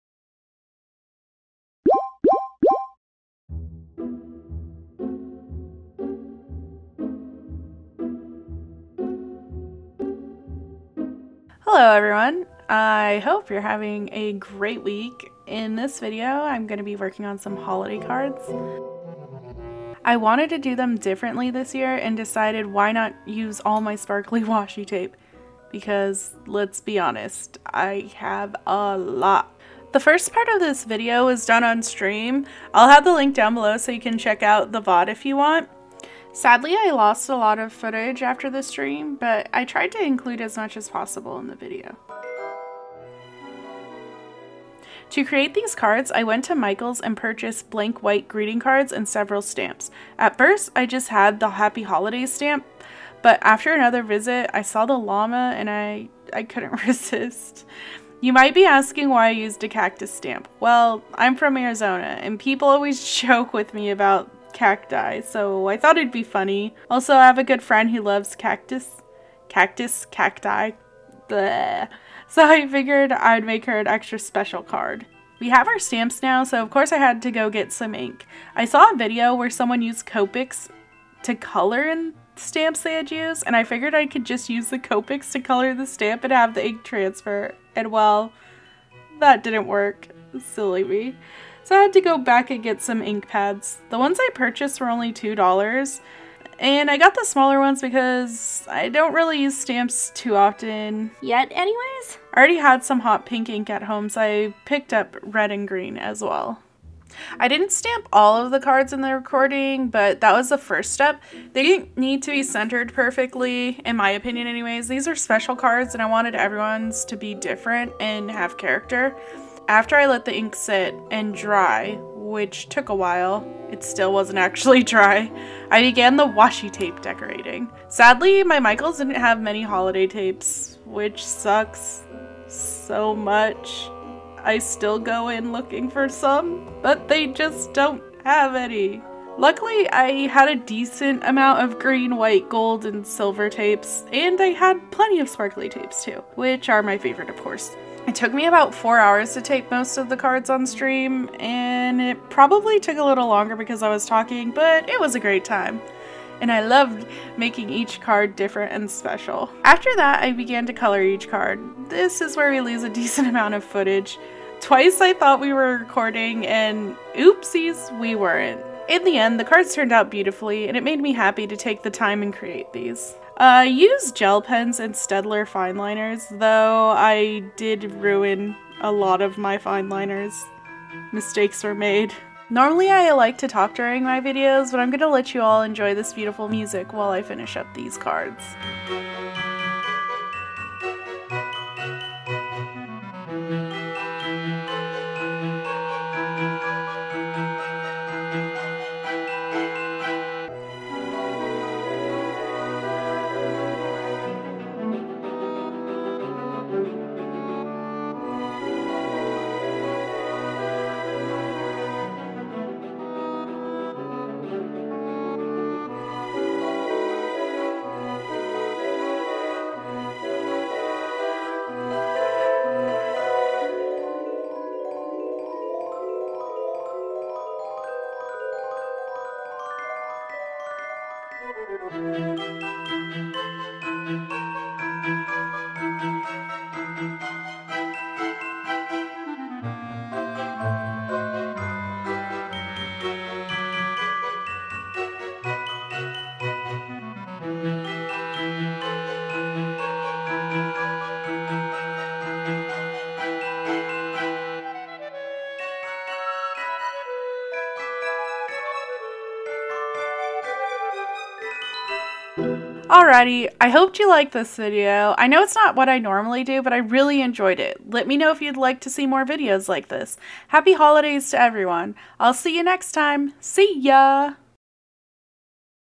❤MAKING HOLIDAY CARDZZZ--2018❤
❤Happy Holidays❤

Hiya! In this video I talk about my process while making my holiday cards for 2018! I hope you enjoy!

PRODUCTS USED:
❤ Staedler Fineliners
❤ Tanmit Gel Pens
❤ Uni-Ball Signo Gel Pen (0.7mm)